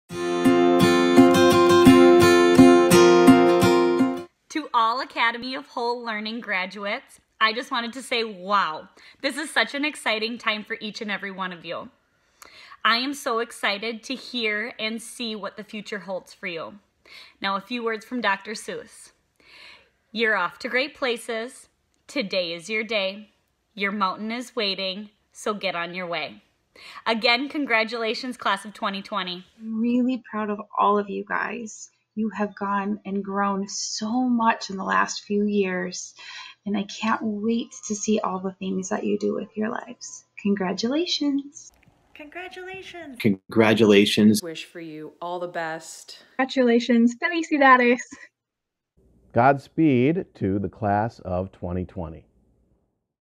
Congrats Class of 2020!
Every Academy of Whole Learning staff member is thrilled and beyond proud of each of our eight graduating seniors. Congrats on all your hard work!